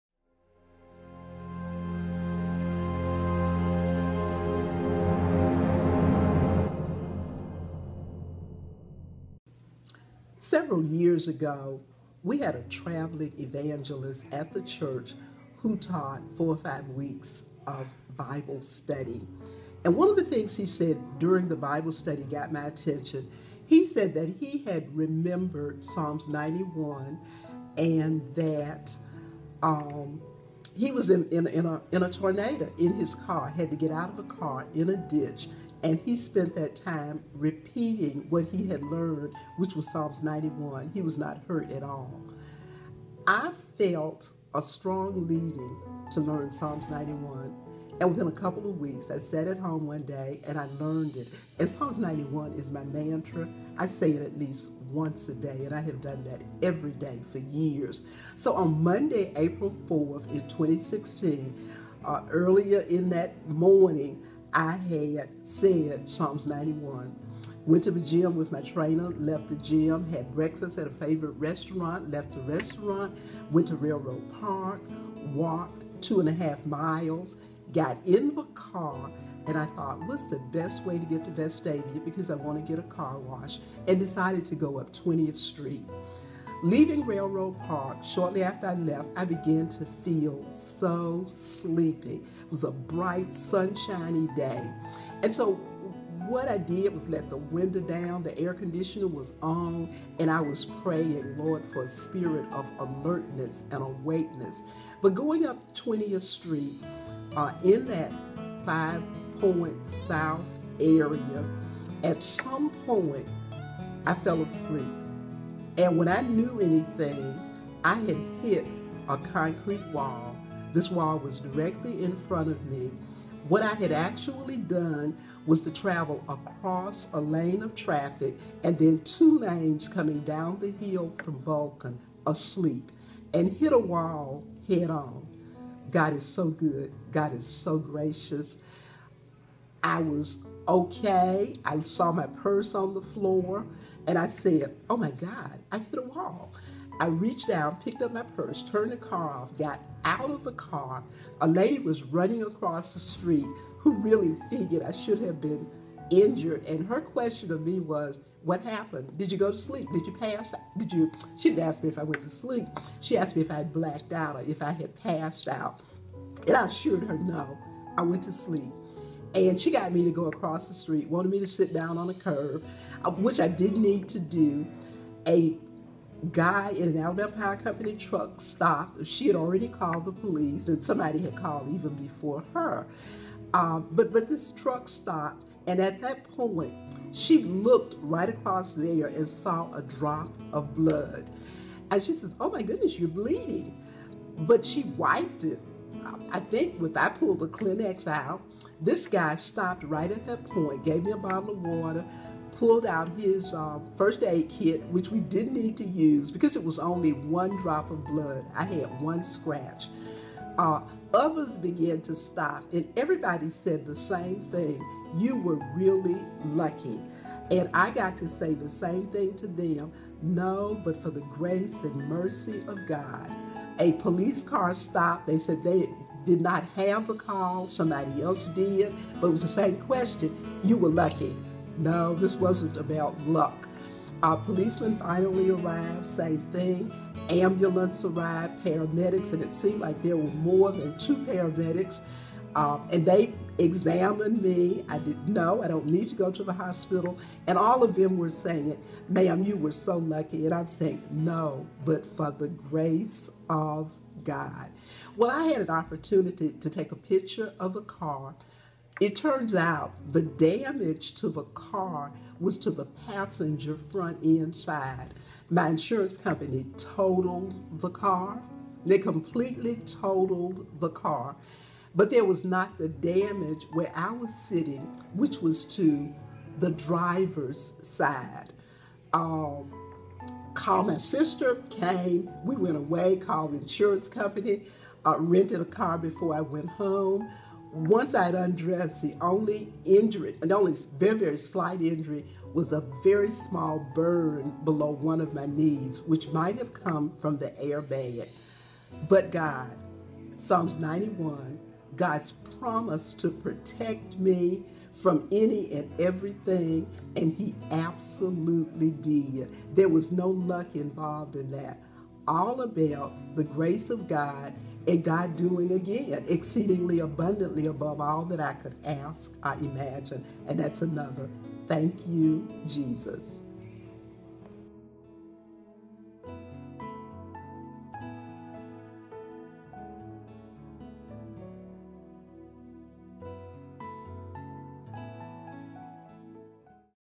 Not Luck, But God!
She was considered lucky by all that saw the state of her car as a result of a crash into a  concrete wall. However, her meditation for years has been on Psalms 91 which is the assurance of the safety of God in a crisis. Her words are, "Not luck, but for the grace of God!"